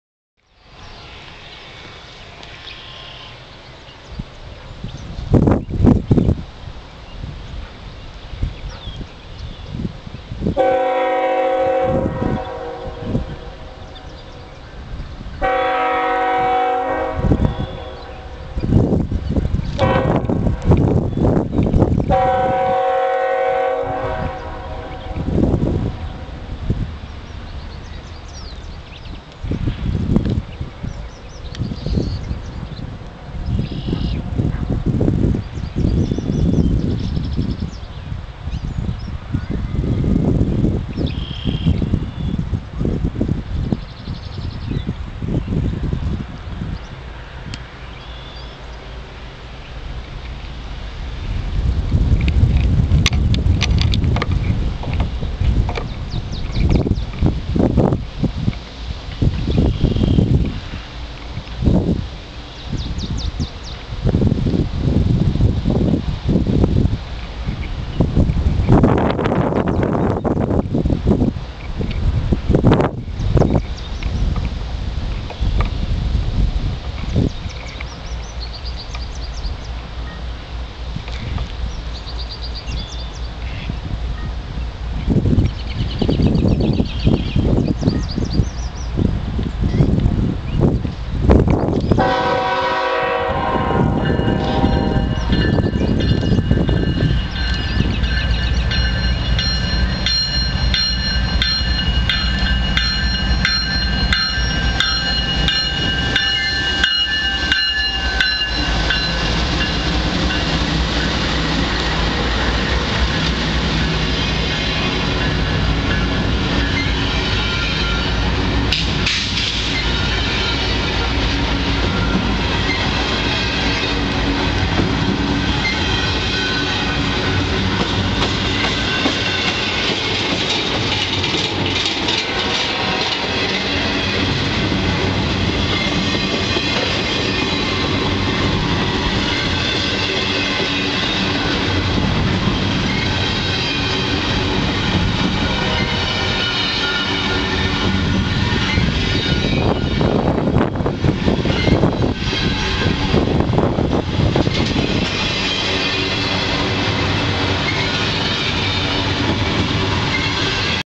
BNSF 7204 West - Crest Hill, IL
BNSF ES44DC 7204 leads a westbound baretable train through Crest Hill.